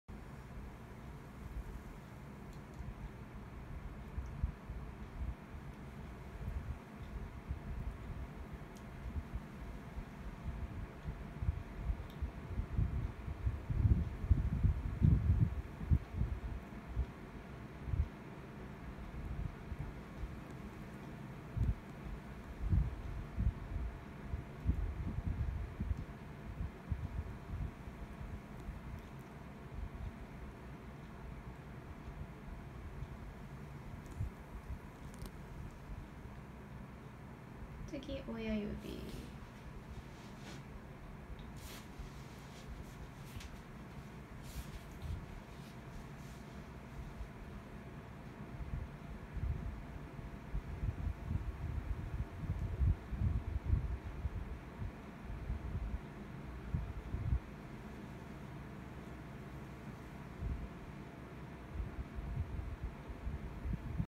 新人メイちゃんマッサージ講習
こちらの講習の動画もどうぞ

広島 性☆感エステ「loving touch（ラビング タッチ）」のオイルマッサージの講習風景です